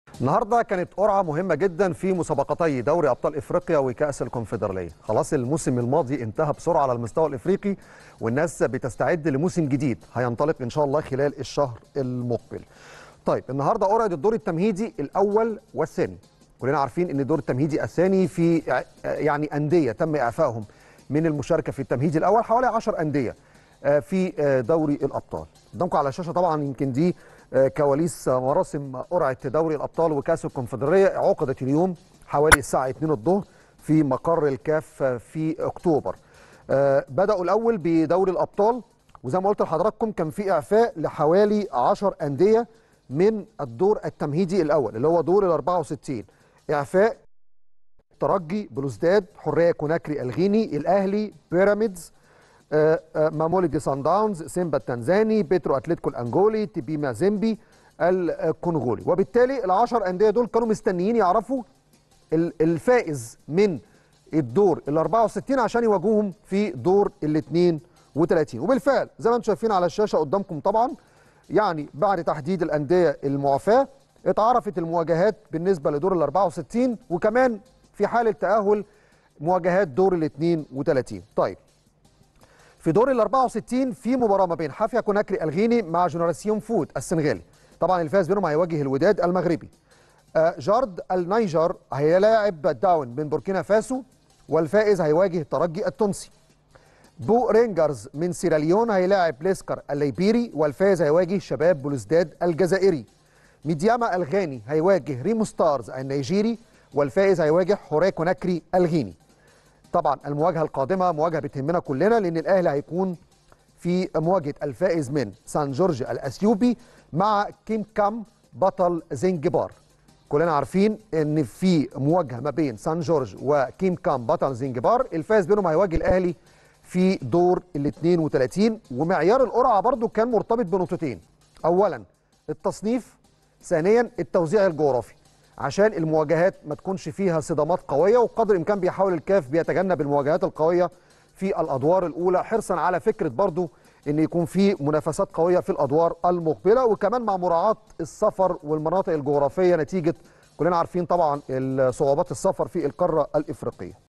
الأهلي حامل اللقب يواجه في دور الـ 32 لدوري أبطال إفريقيا الفائز من مواجهة "KMKM" بطل زنجبار
الأهلي حامل اللقب يواجه في دور الـ 32 لدوري أبطال إفريقيا الفائز من مواجهة "KMKM" بطل زنجبار من سان جورج الإثيوبي